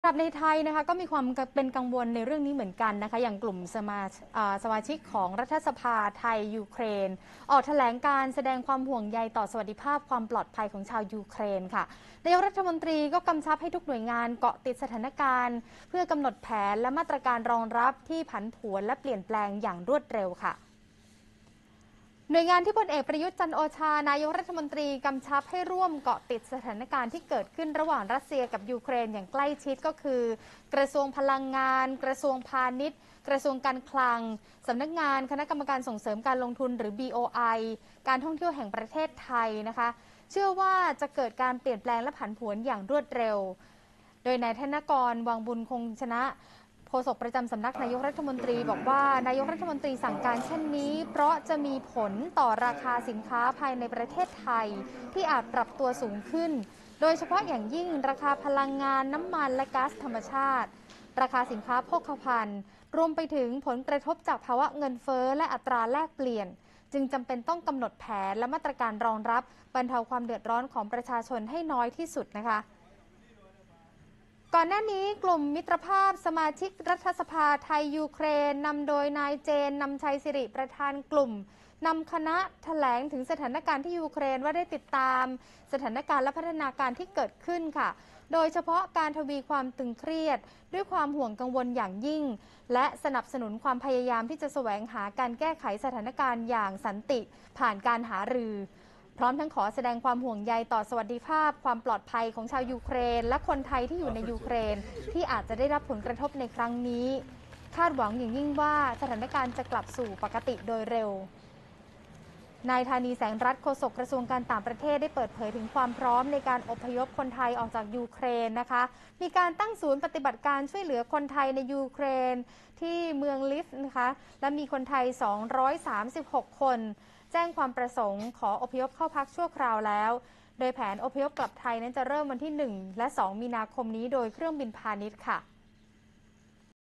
นายกฯ สั่งเกาะติดสถานการณ์ยูเครน (26 ก.พ. 65)
กลุ่มมิตรภาพสมาชิกรัฐสภาไทย-ยูเครน ออกแถลงการณ์แสดงความห่วงใยต่อสวัสดิภาพความปลอดภัยของชาวยูเครน ขณะที่นายกรัฐมนตรี กำชับให้ทุกหน่วยงานเกาะติดสถานการณ์ เพื่อกำหนดแผนและมาตรการรองรับที่ผันผวนและเปลี่ยนแปลงอย่างรวดเร็ว